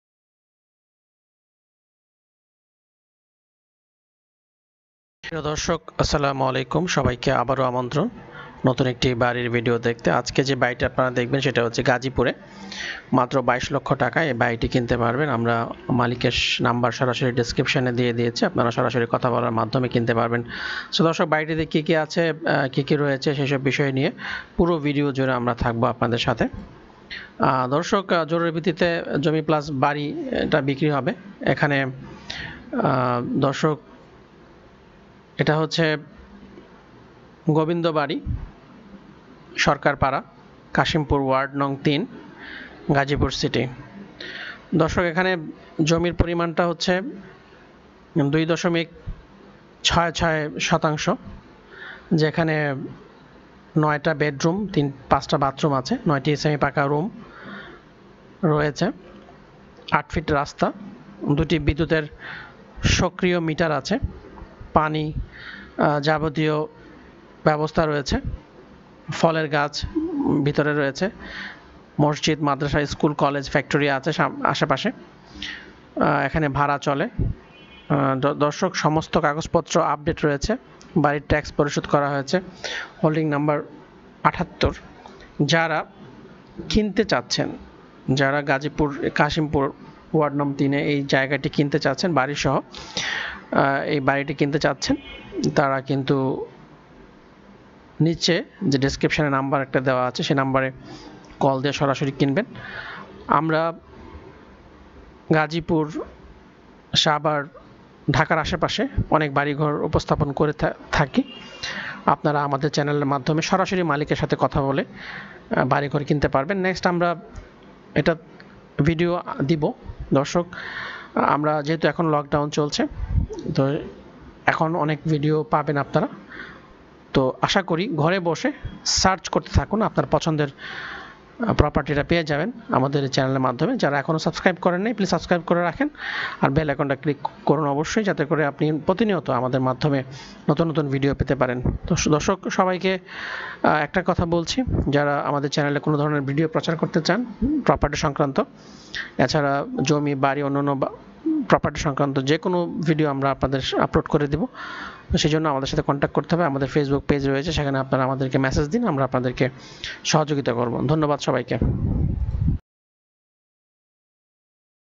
মাত্র ২২ লাখ টাকায় গাজীপুরে দারুন রেডি ১টি রেডি বাড়ি বিক্রয় হবে ।। ready house sale in gazipur
২.৬৬ শতাংশ  জমি  উপর  ১  তলা রেডি বাড়ি বিক্রয় হবে যার মূল্য   ২২  লাখ   টাকা মাত্র

যোগাযোগ নাম্বারঃ